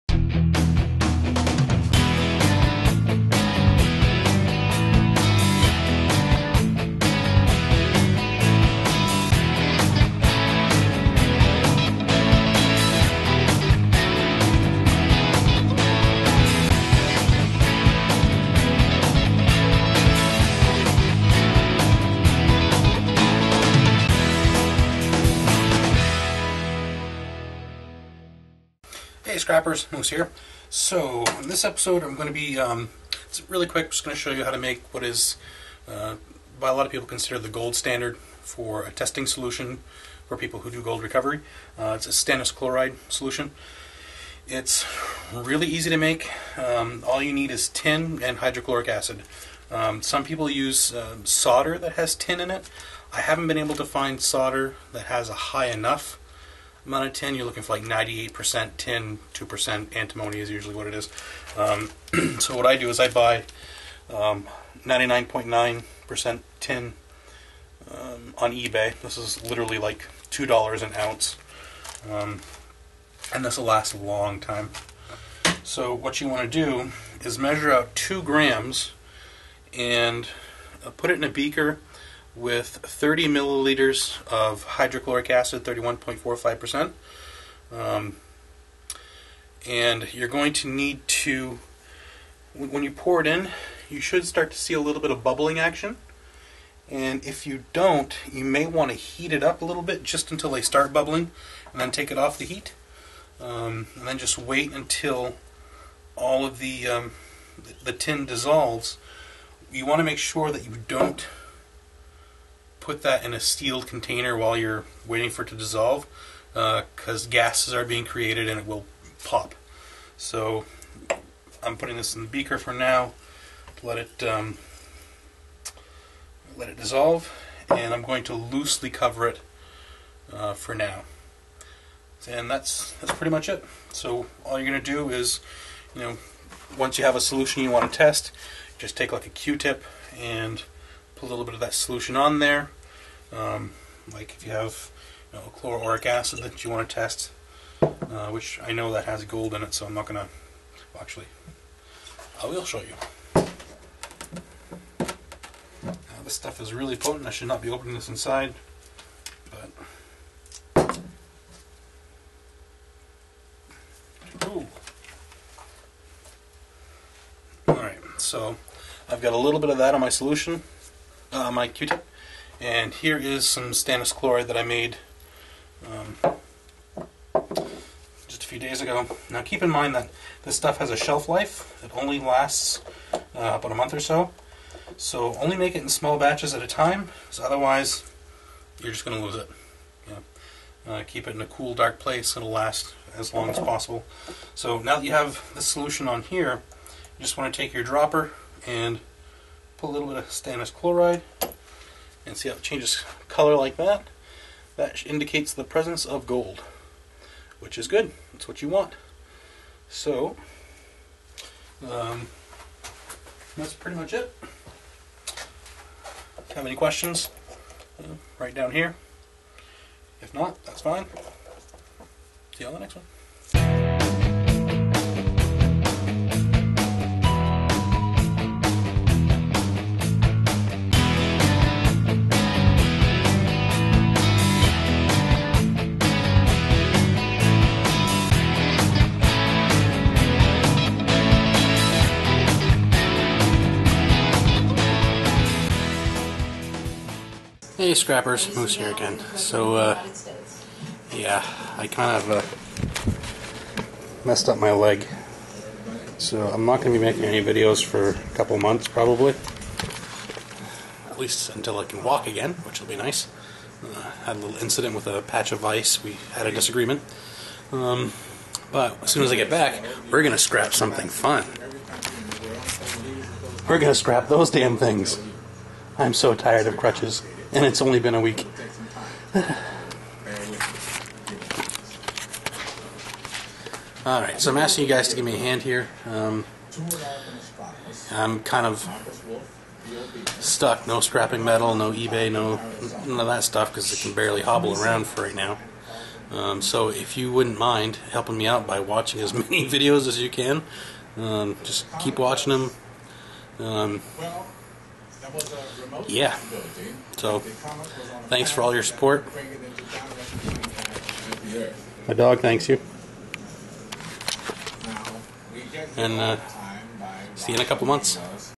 How To Make Stannous Chloride for Testing Gold Solutions
Easy process for making this gold standard (no pun intended) test
solution.  Remember to NOT cap the bottle while the tin is dissolving!